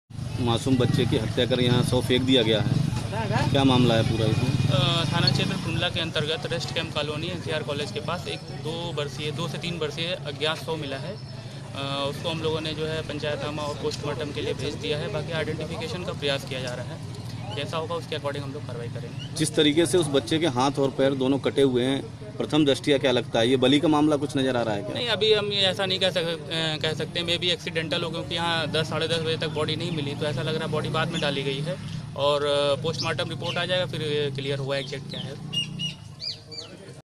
जनपद फिरोजाबाद के थाना टूण्डला क्षेत्र में 02 वर्षीय बालक के मिले शव
जनपद फिरोजाबाद के थाना टूण्डला क्षेत्र में 02 वर्षीय बालक के मिले शव के सम्बन्ध में क्षेत्राधिकारी टूण्डला की बाईट